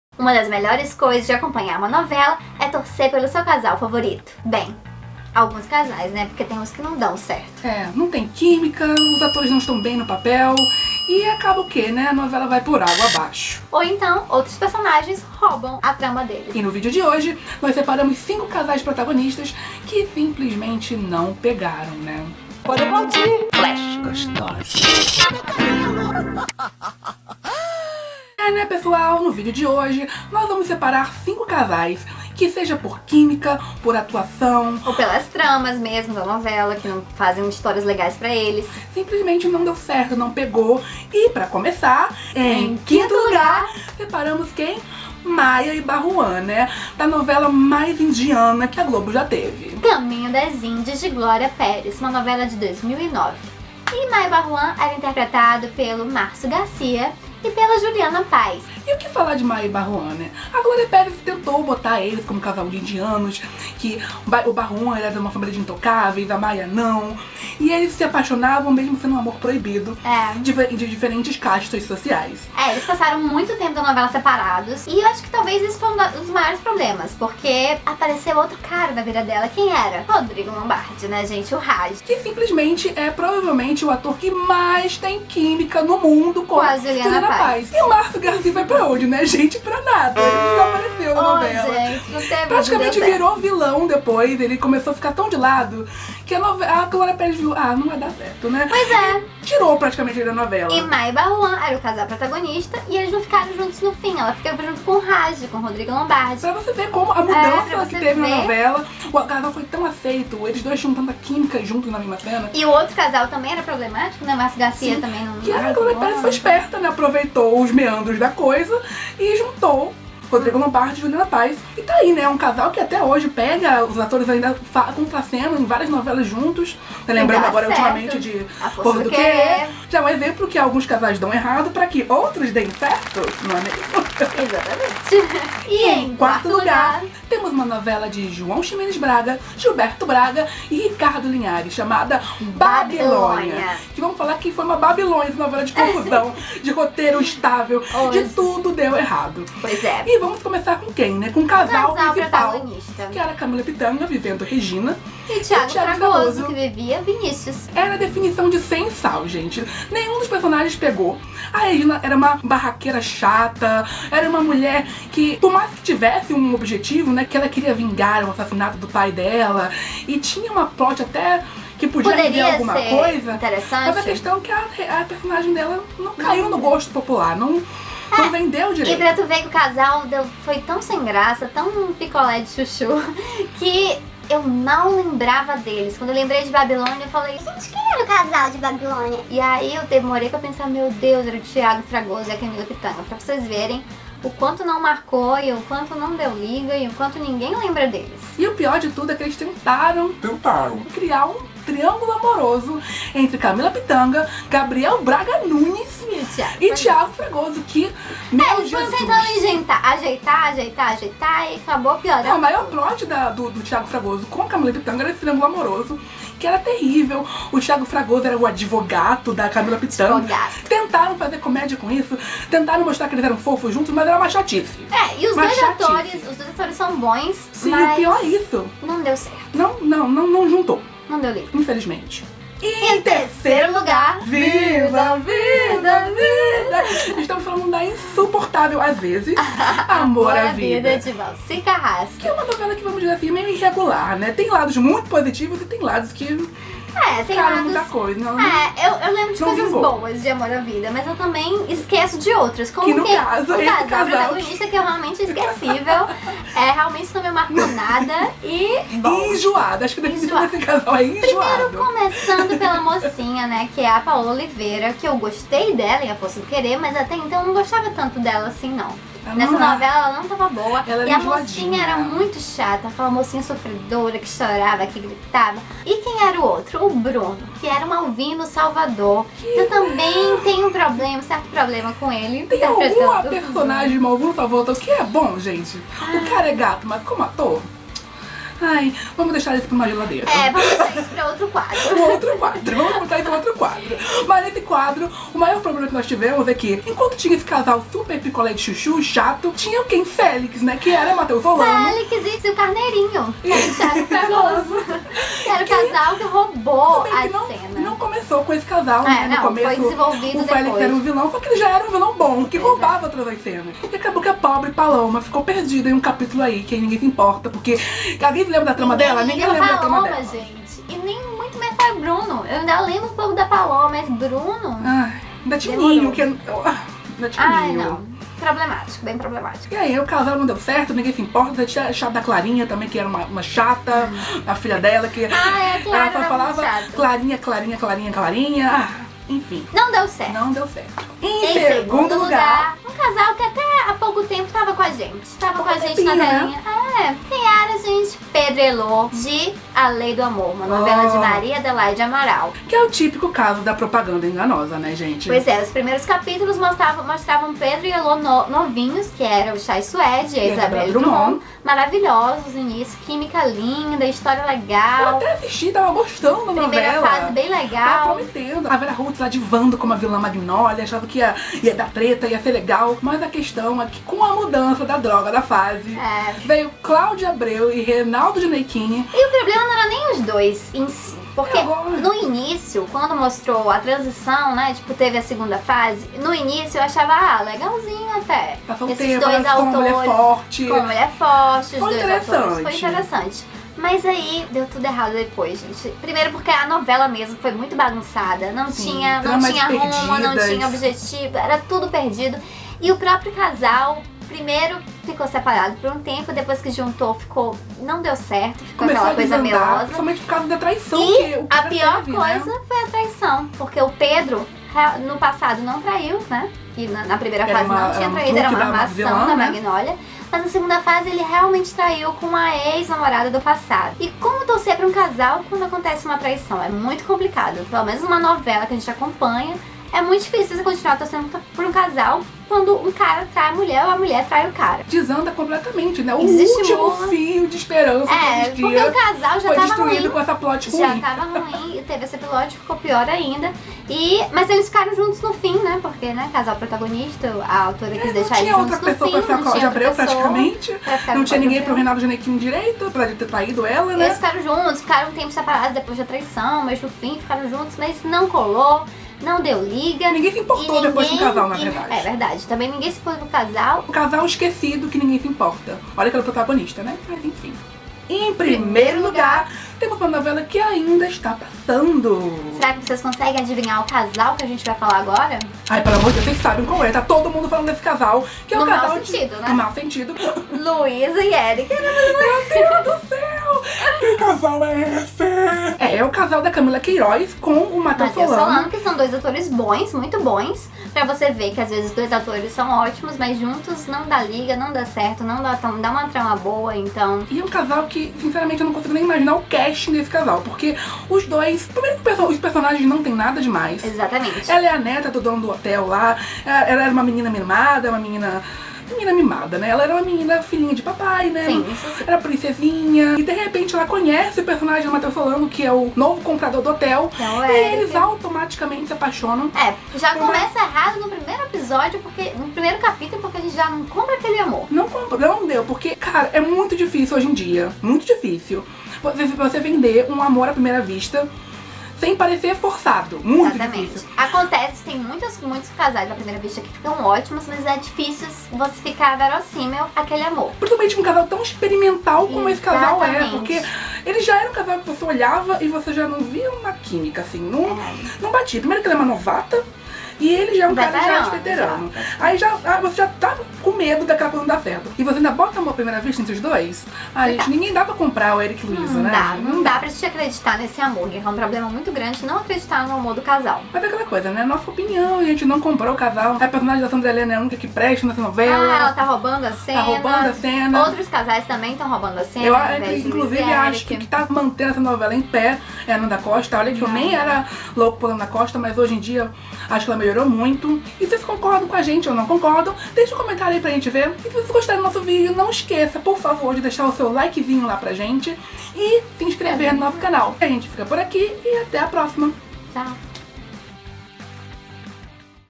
5 CASAIS de NOVELA que NÃO DERAM CERTO
Novela que é novela tem sempre um casalzão daqueles para você torcer! Mas algumas vezes, o casal simplesmente não vinga! No video de hoje, analisamos 5 casais de novelas da Globo que não caíram no gosto popular!